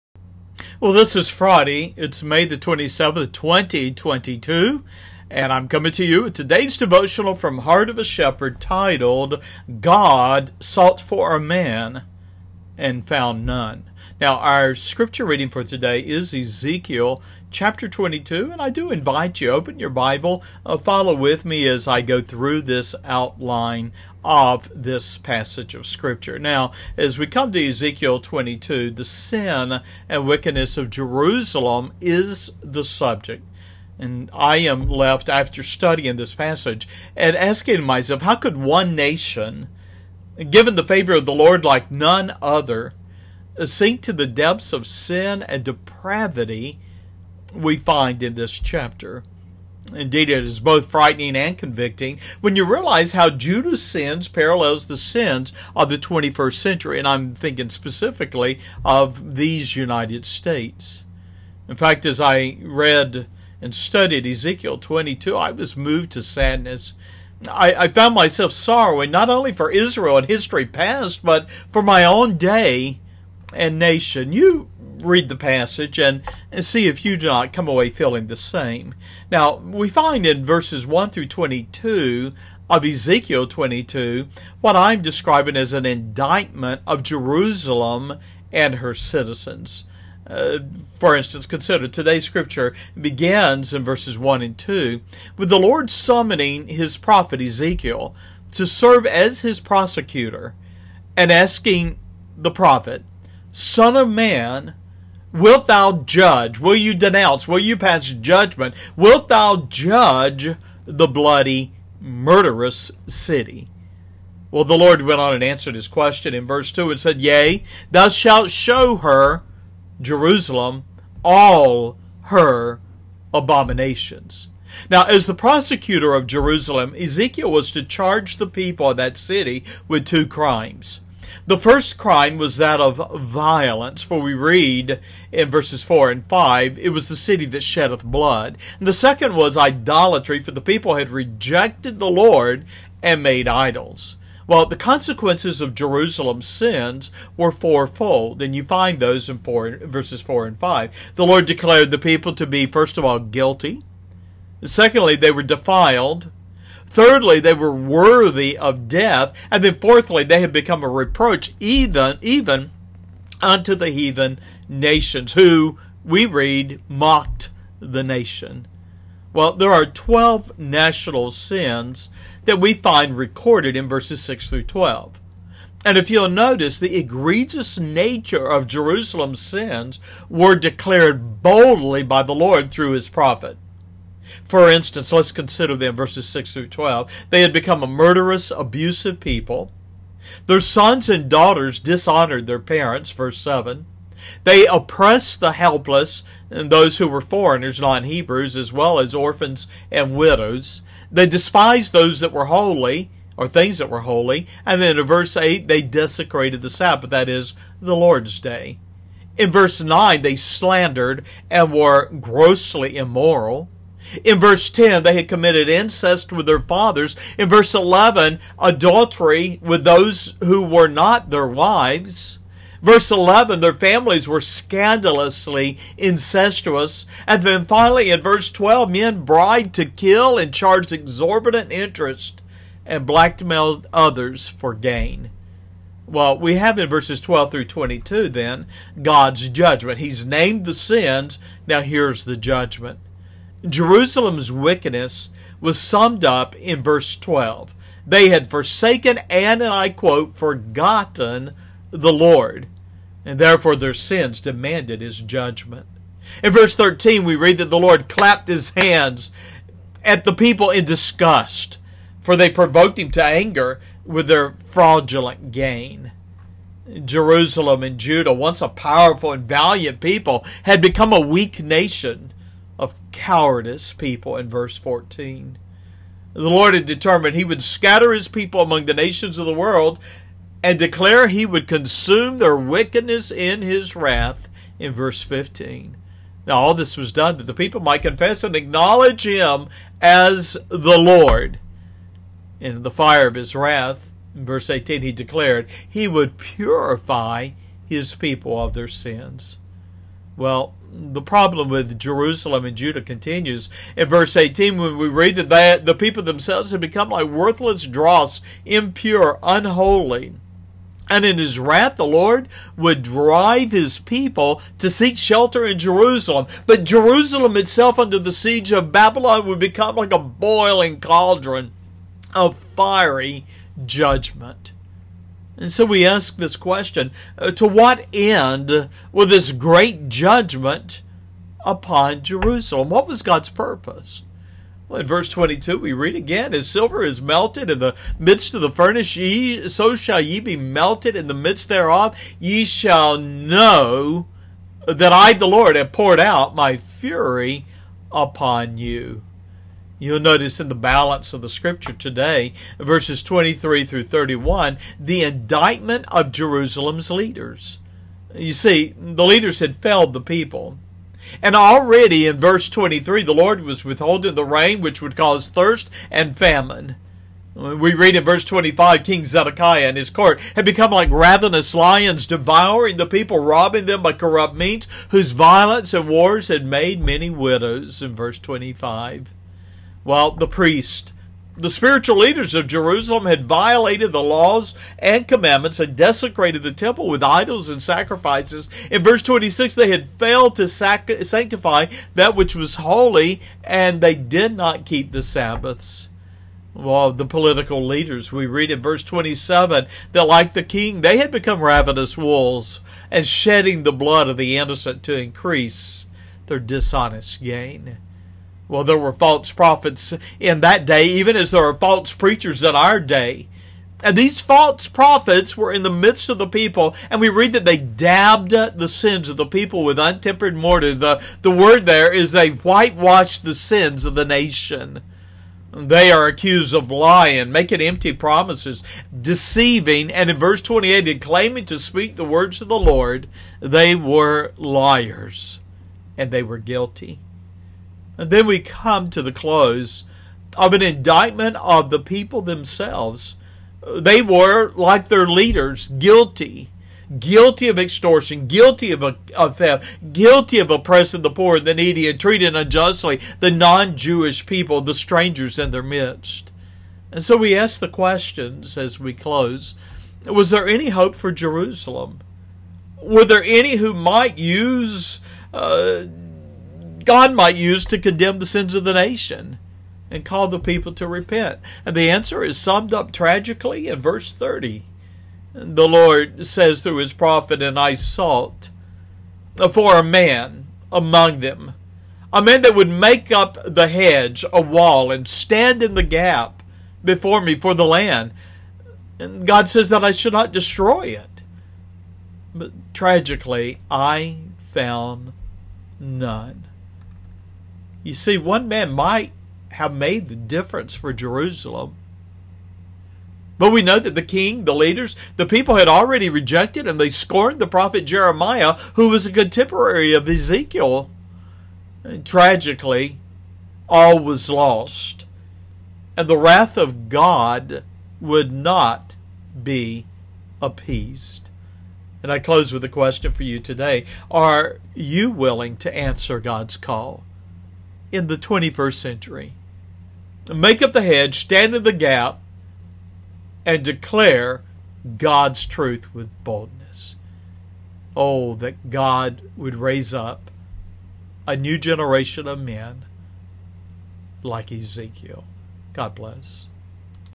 God Sought for a Man, and Found None (Ezekiel 22) – A daily devotional.
How could one nation, given the favor of the LORD, sink to the depths of sin and depravity? I was moved to sadness as I studied Ezekiel 22, for I found myself sorrowing not only for Israel in history past, but for my own day and nation. One man might have made the difference for Jerusalem; but the king, the leaders, and the people had rejected and scorned Jeremiah. Tragically, all was lost and the wrath of God would not be appeased (22:31).